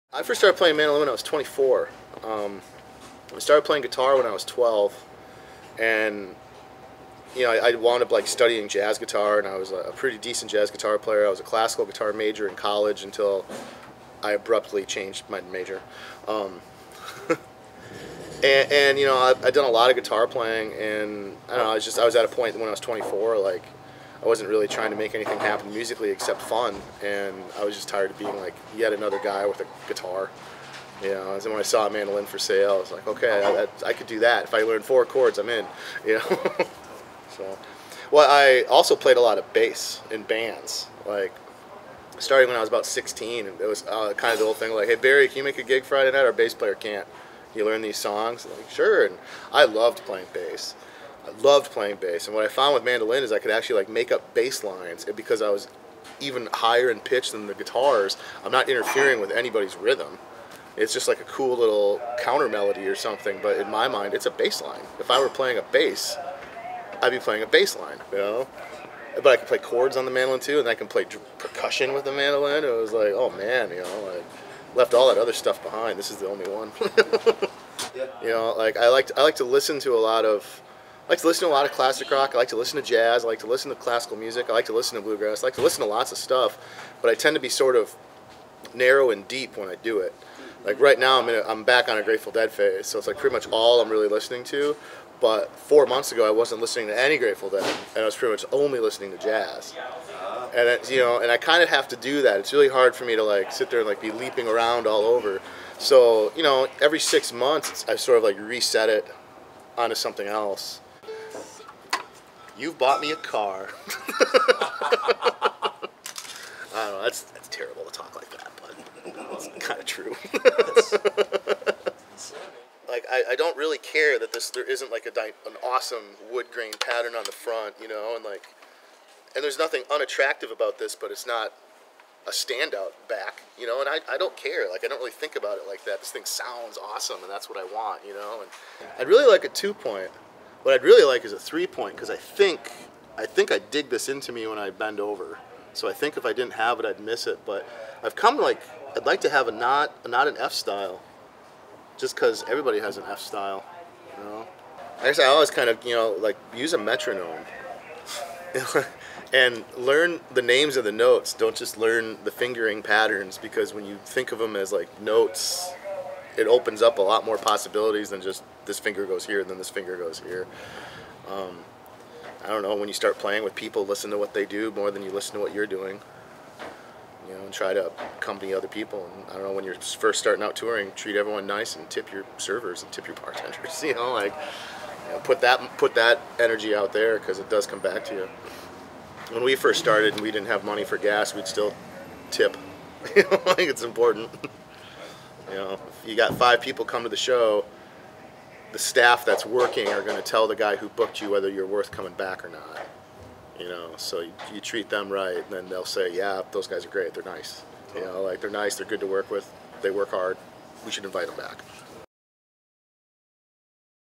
Weber Mandolins: Artst Interview with Erik Berry | Trampled by Turtles
In this short video interview with Erik Berry of Trampled by Turtles, Weber Mandolins discusses topics such as musical influences, why Erik plays the mandolin and advice for aspiring musicians. Filmed on location at the Crystal Ballroom in Portland, OR 2015.